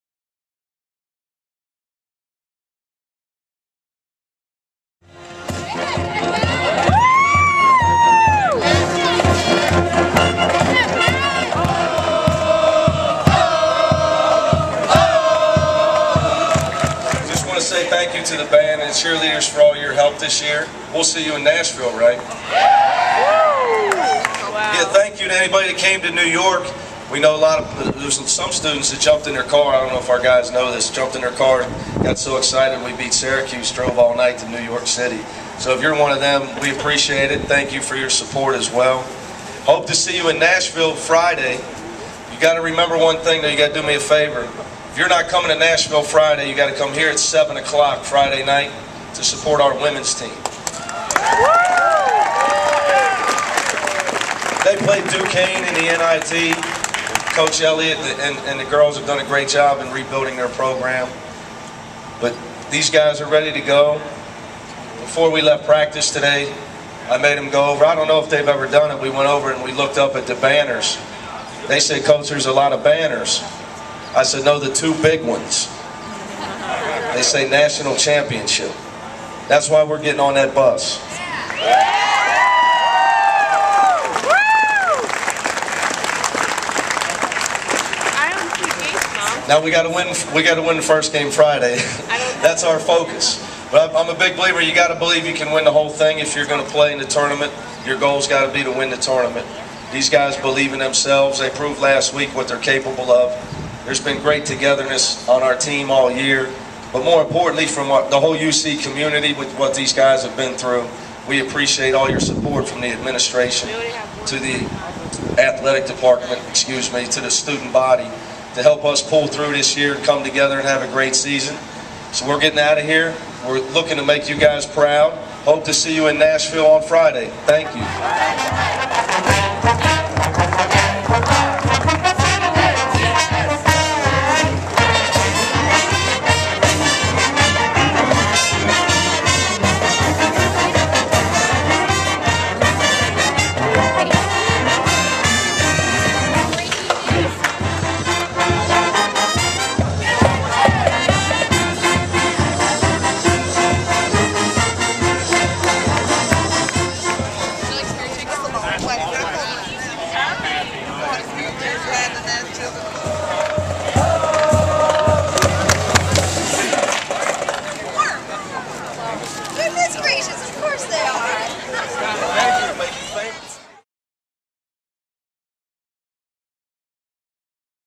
UC Bearcats Send-Off
Head Coach Mick Cronin thanks the fans who turned out to send off the
UC men's basketball team to the NCAA tournament.